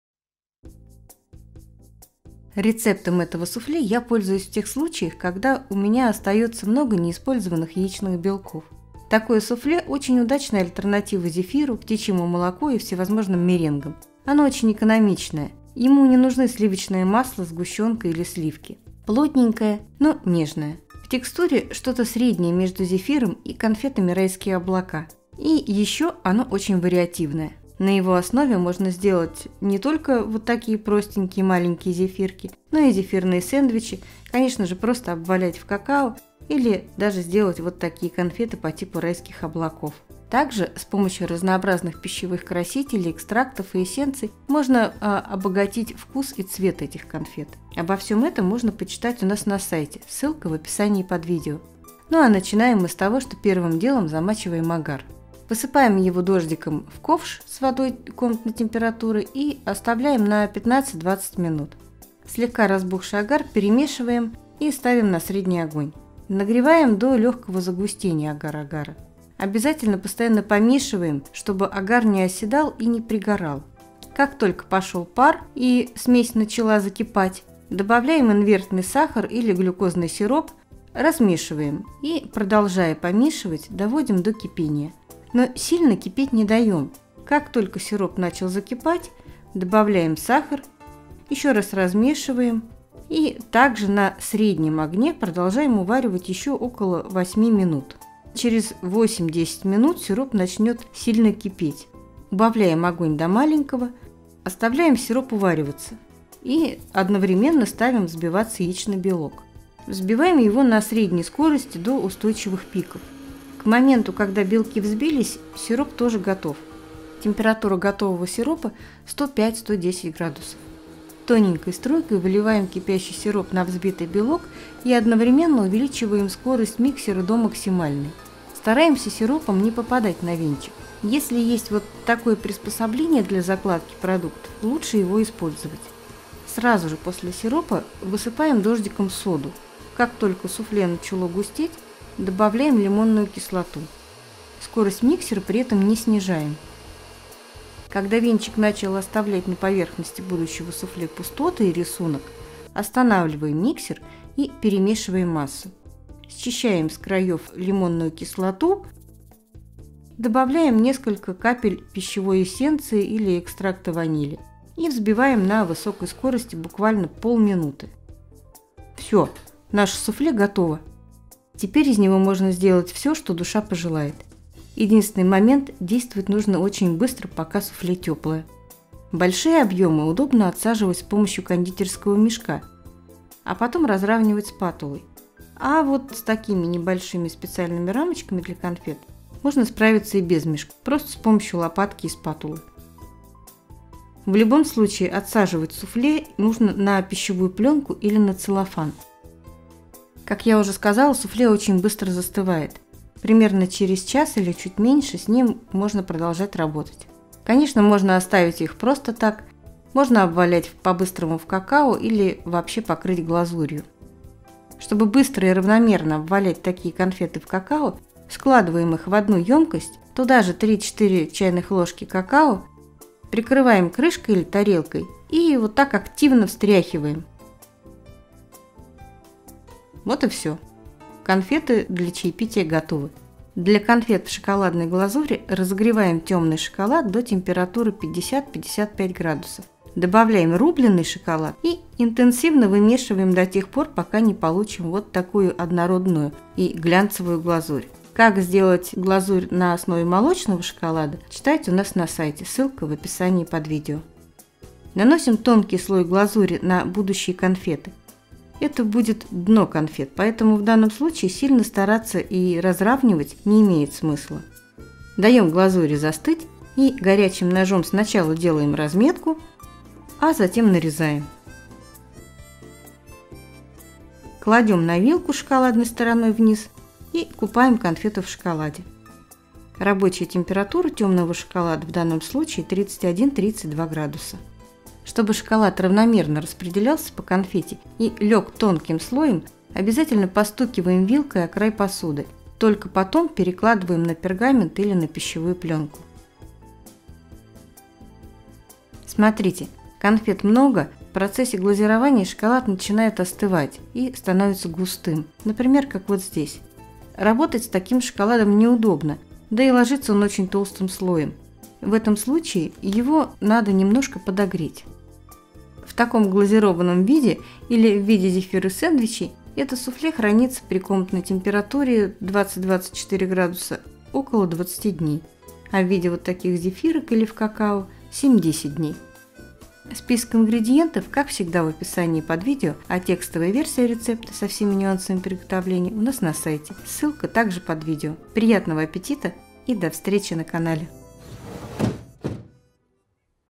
Домашнее суфле а-ля конфеты "Райские облака". Без сливочного масла и сгущенки
Это простое и быстрое в приготовлении суфле делается только на основе яичного белка и сахара. Здесь не нужны сгущенка или сливочное масло. Суфле получается плотным, нежным и напоминает по вкусу конфеты «Райские облака». Рецепт не сложный и вариативный: можно сделать глазированные конфеты, небольшие пирожные или просто обвалять конфеты в какао.
Глазированные шоколадом или в виде зефирных сэндвичей конфеты хранятся без холодильника около 20 дней. Без глазури — 7-10 дней.
Рецепт времен 80-90х гг., с какого-то из кондитерских производств, случайно попал ко мне в тетрадку и так там и остался: для меня он просто идеальная альтернатива меренгам, зефиру и прочим рецептам на основе белков. Особенно, когда после праздников нужно куда-то деть яичные белки.
По этому рецепту получается чуть больше одного килограмма суфле (без глазури).
ИНГРЕДИЕНТЫ:
Для суфле
120 г яичного белка
270 г воды комнатной температуры
13 г агар-агара силой 1200 блум (или17-18 г силой 900 блум)
560 г сахара
180 г любого инвертного сахара
3 г пищевой соды
5 г лимонной кислоты
5-10 капель эссенции (в зависимости от насыщенности) вишневой косточки, клубничной, сливочной и т.д.) или 1-2 ч.л. экстракта ванили
Для темперированного шоколада (рецепт шоколадной глазури в примечании, ниже)
350-400 г темного шоколада (разделить на 70% и 30%)

Подробное пошаговое описание рецепта со всеми нюансами и примечаниями на нашем сайте
Успехов вам в ваших кулинарных практиках и экспериментах!
Питайтесь вкусно и разнообразно😋 Мирной жизни, хорошего настроения и приятного аппетита! Спасибо за внимание 😊💖